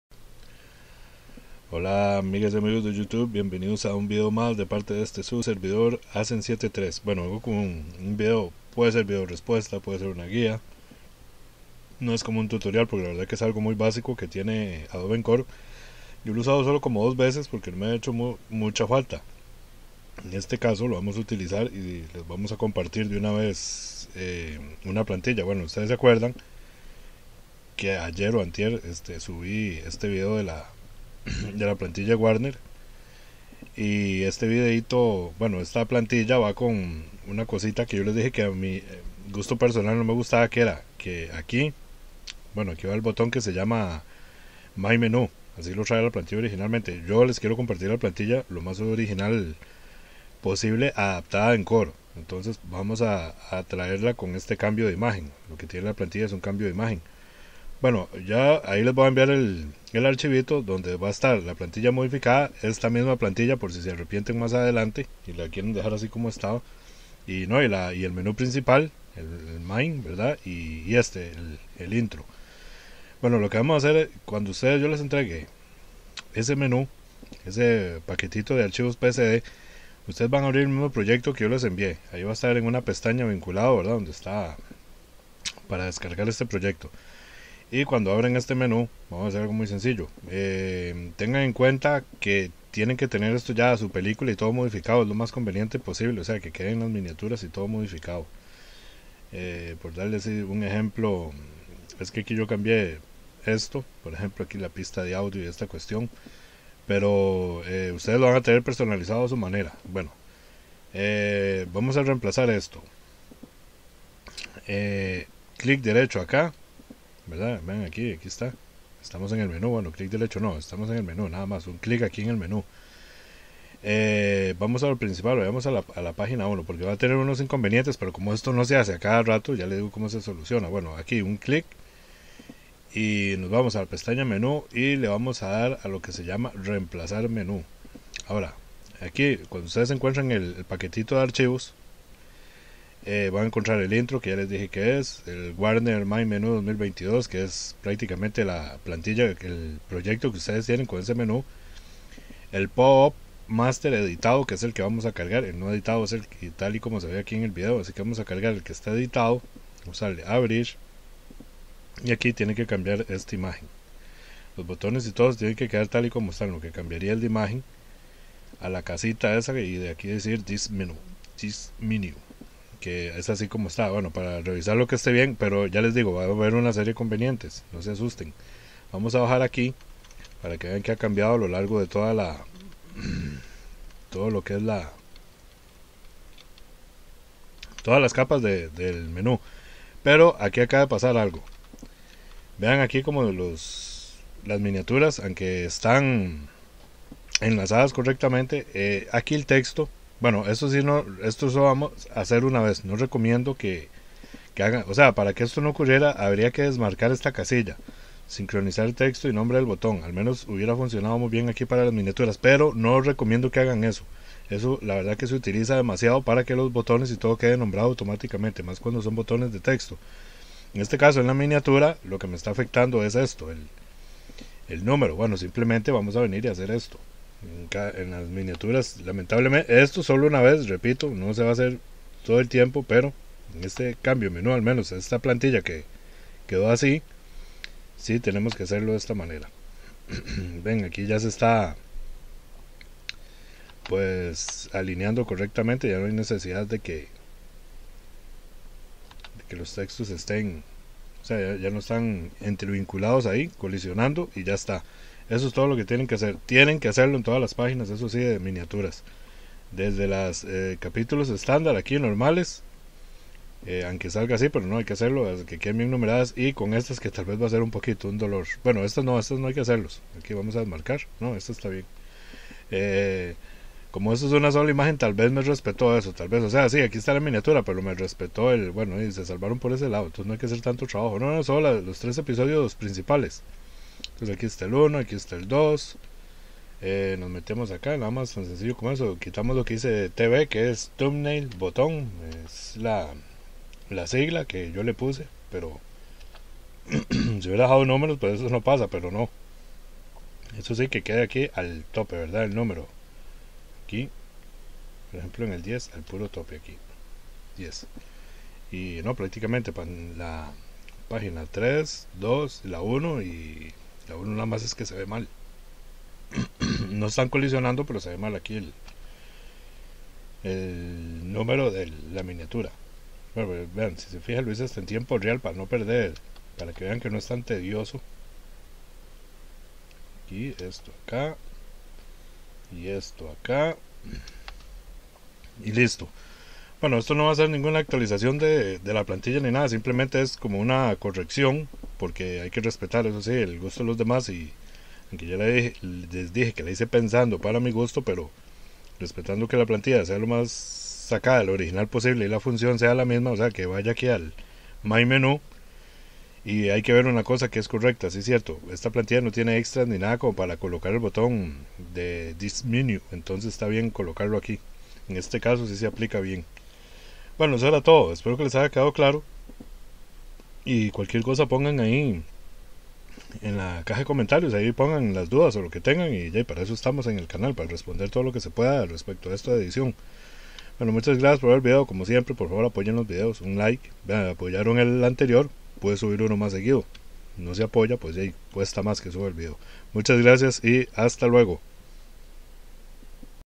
Encore: Reemplazar menú Blu Ray
Bueno de una en esta guía, les entrego las plantillas del menú Warner y además corregida el PopUp con el botón "Disc menú" .
Link, aprovechen por que si lo eliminan no hay resubida.